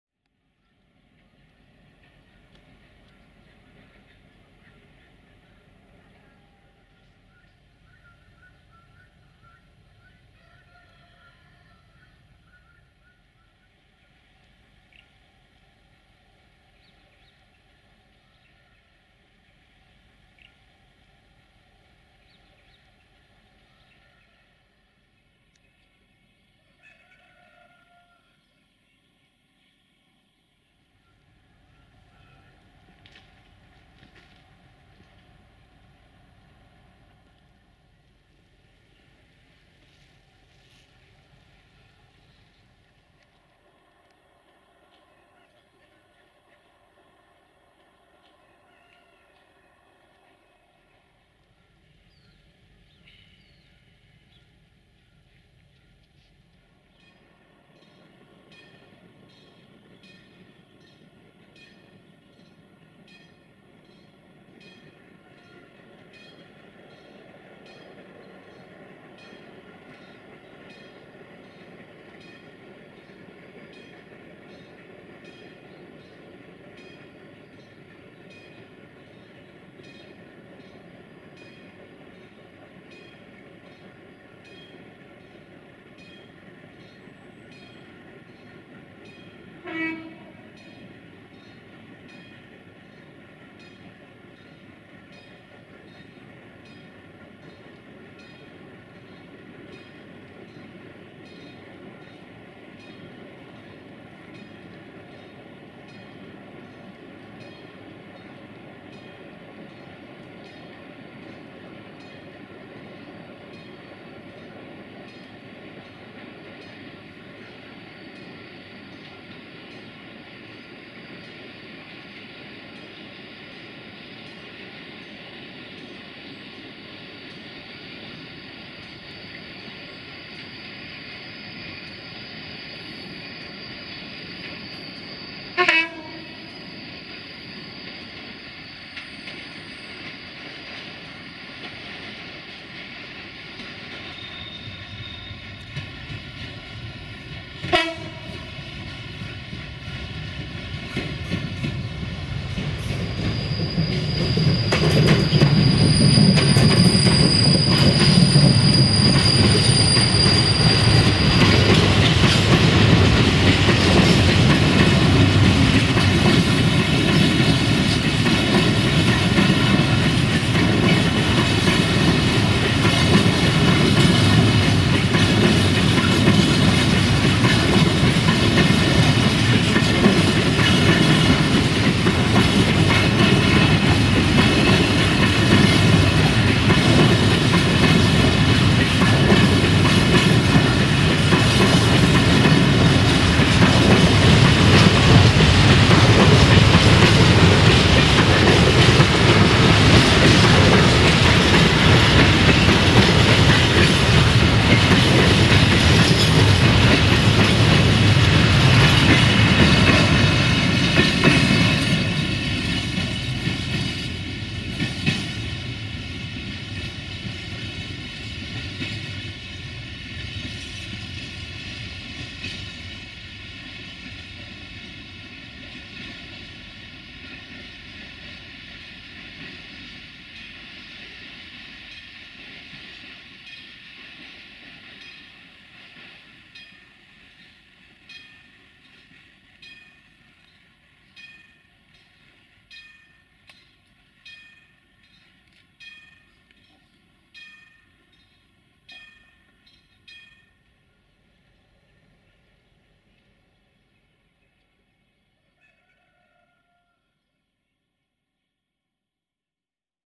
LE5100KW 40-0118-2 & Marfar CFR MARFĂ Freight Train in Gara Boju Station - 06 November 2020
00:59 CFR MARFĂ Freight Train/Marfar/Guterzug - 060-EA/LE5100KW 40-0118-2 RO-SNTFM

06 November 2020